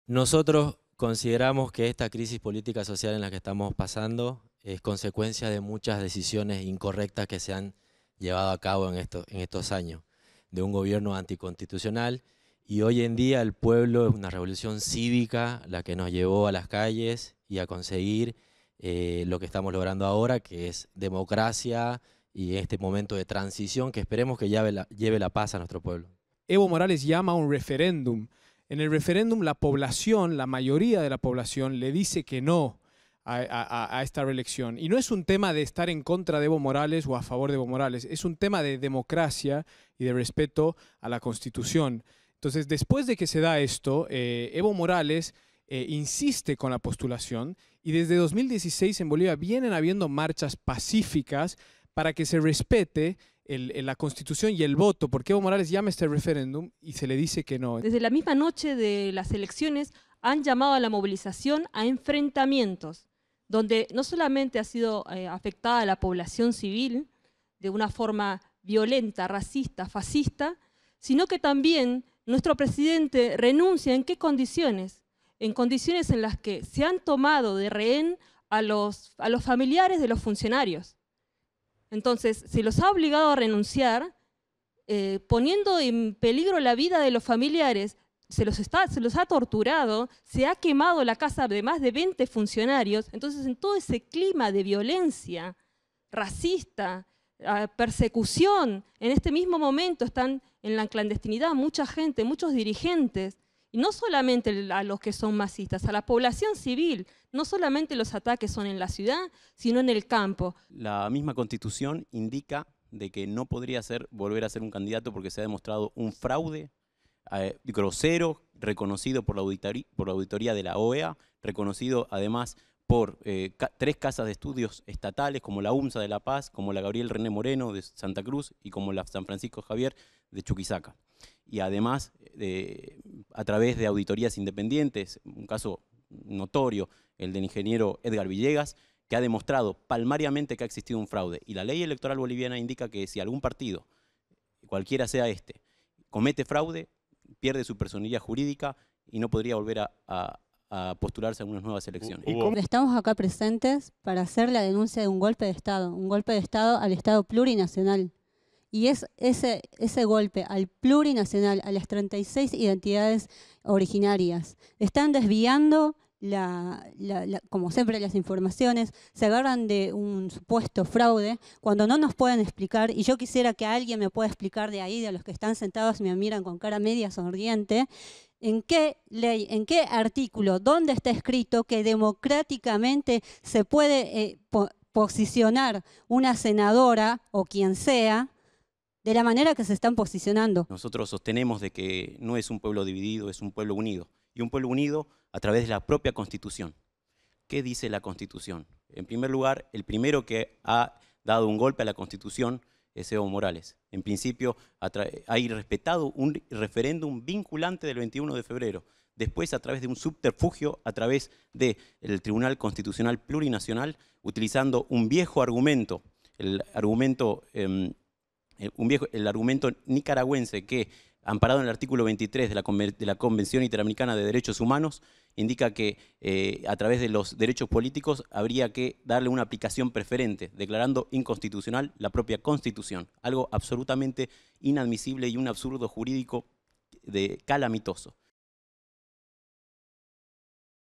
A DOS VOCES | La grieta por Bolivia: miradas antagónicas sobre Evo Morales
Inmigrantes e integrantes de la comunidad boliviana en la Argentina, con miradas antagónicas sobre Evo Morales y la situación que se vive en Bolivia. testimonios a favor y en contra de Evo Morales.